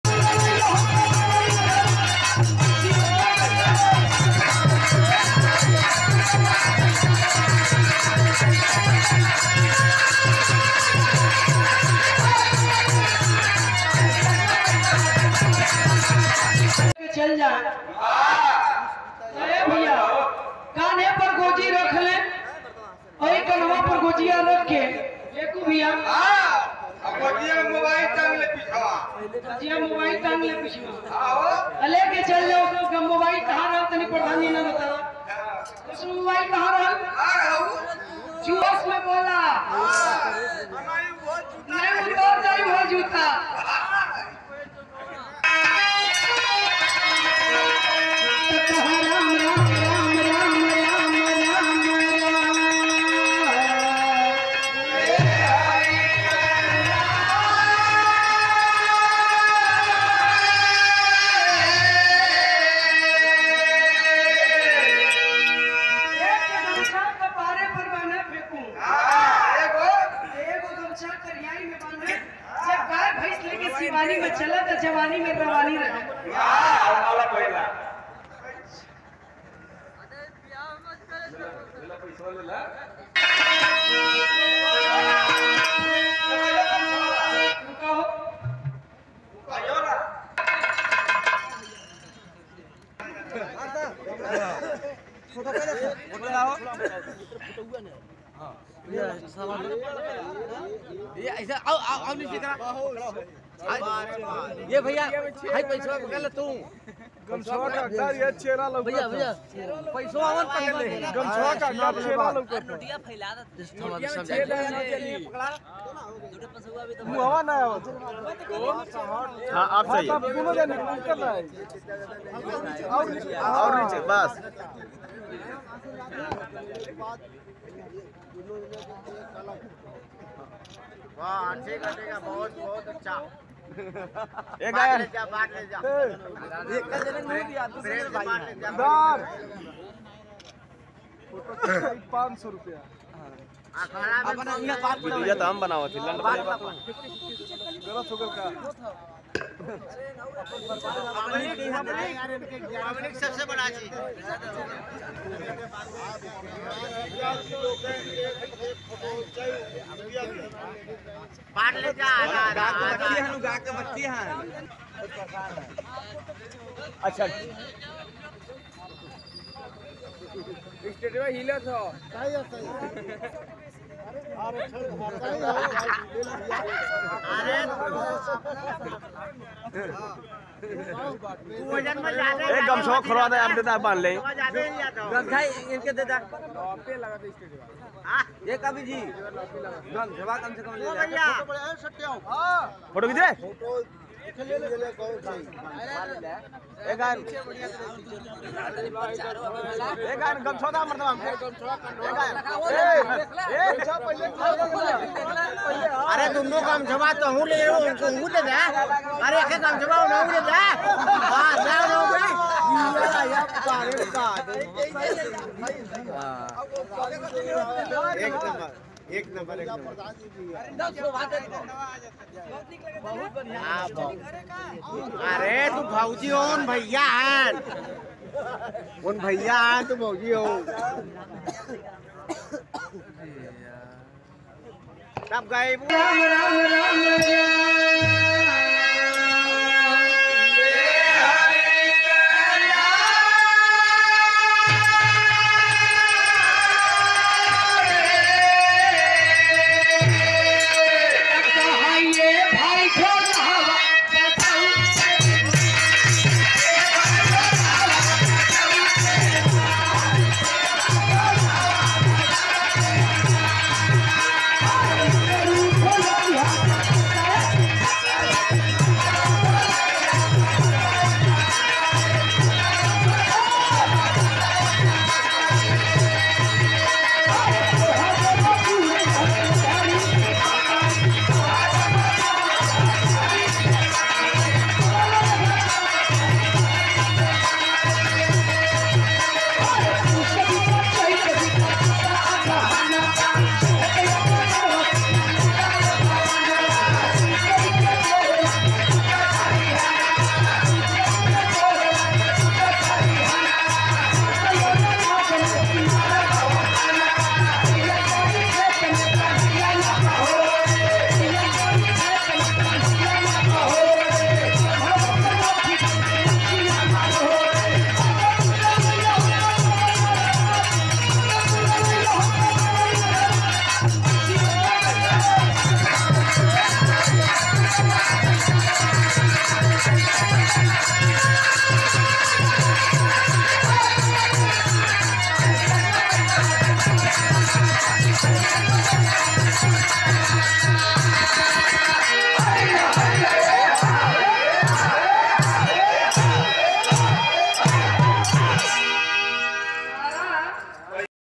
नीतू राज ने अहिरऊवा डांस किया और चनैनी गायी उपेन्द्र लाल यादव जोरदार मुकाबला
नीतू राज ने अहिरऊवा डांस किया और चनैनी गायी उपेन्द्र लाल यादव जबरदस्त मुकाबला